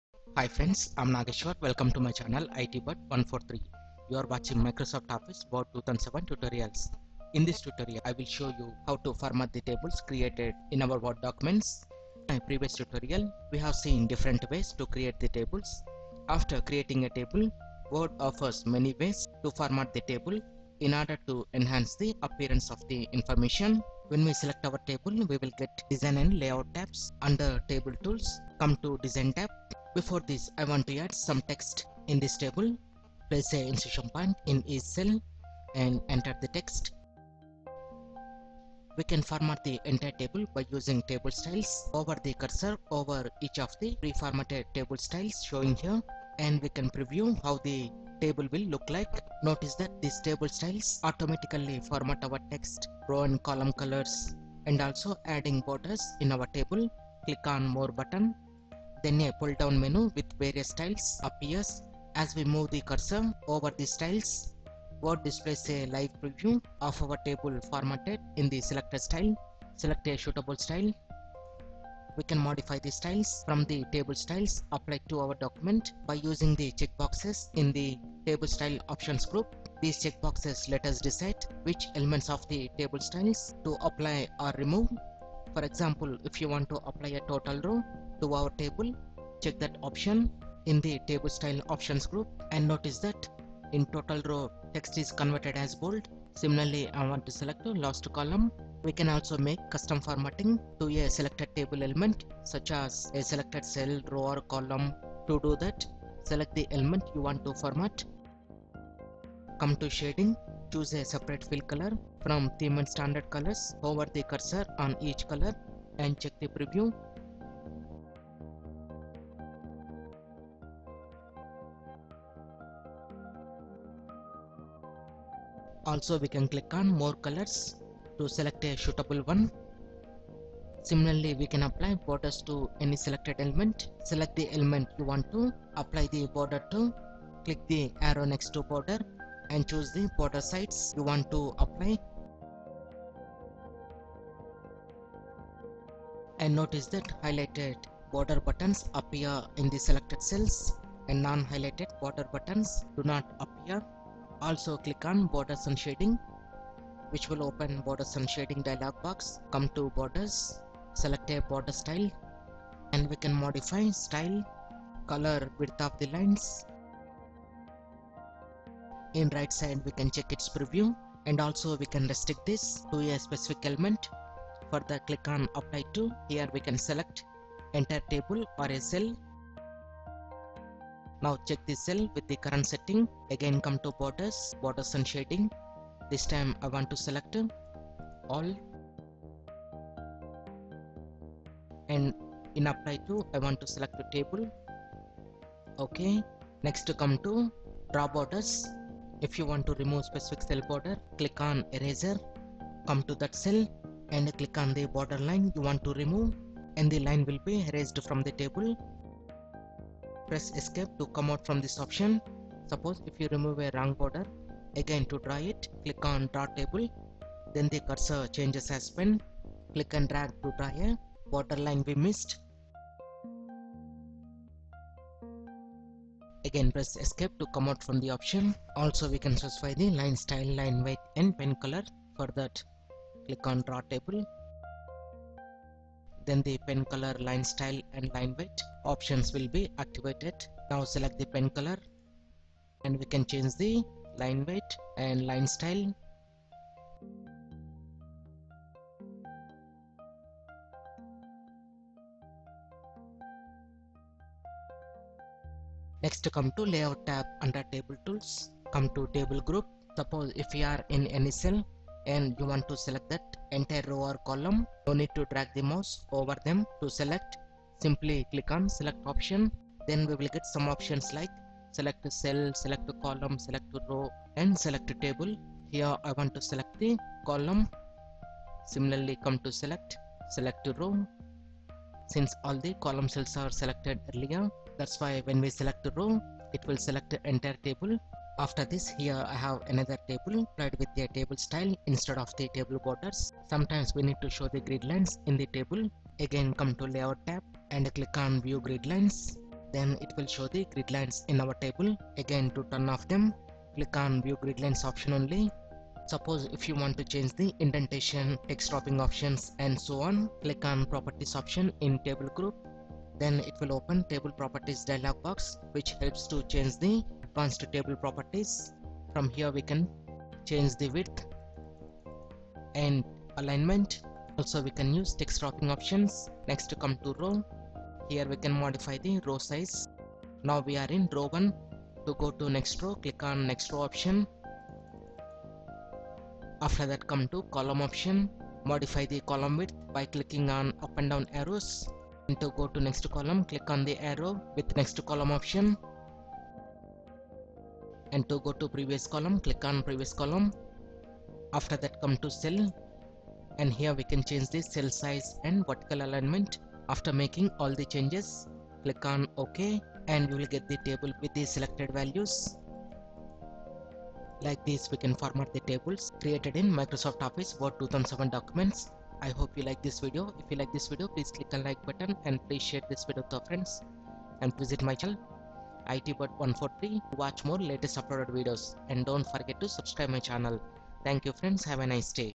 Word 2007: How to format Tables in word (Part – 1)
In this tutorial, I have shown how to format the Tables created in Microsoft Office Word 2007 documents.